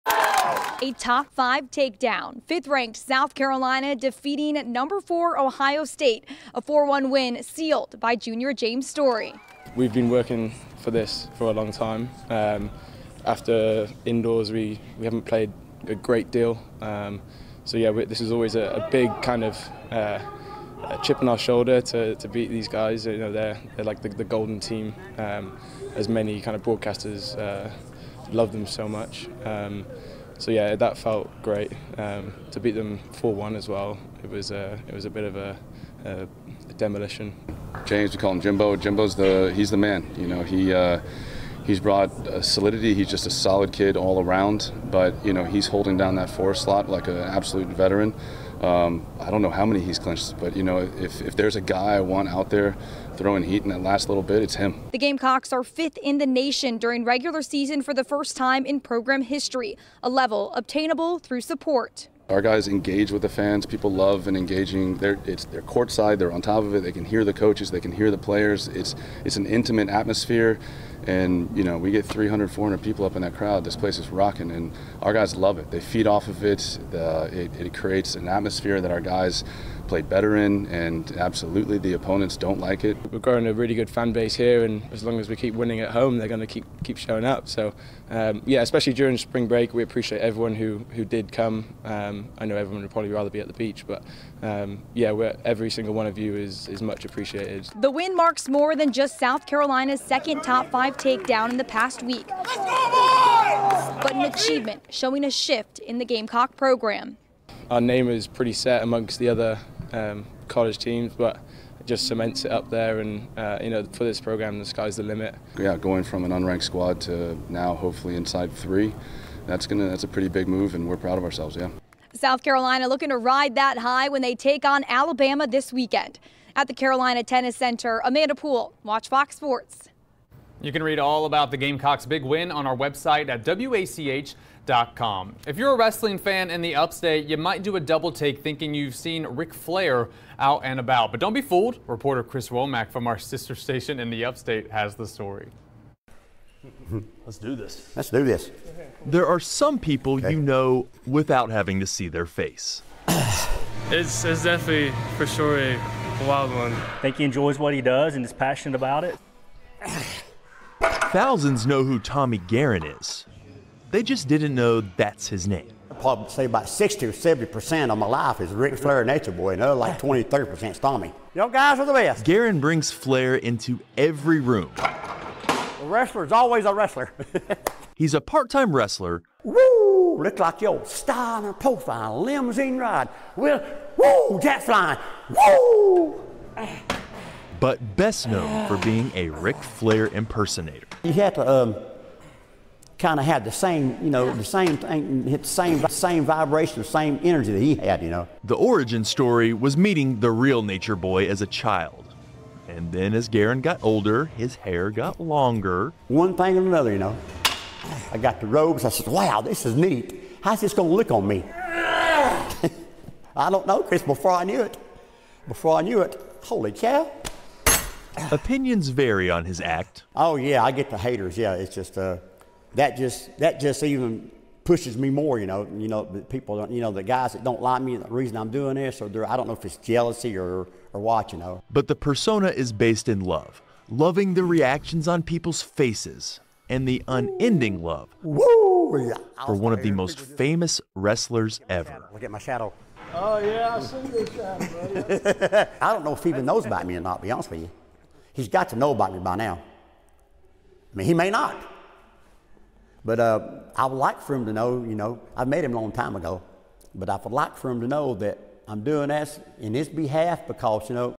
No. 5 Gamecocks upset No. 4 Ohio State in 4-1 'demolition'
South Carolina men's tennis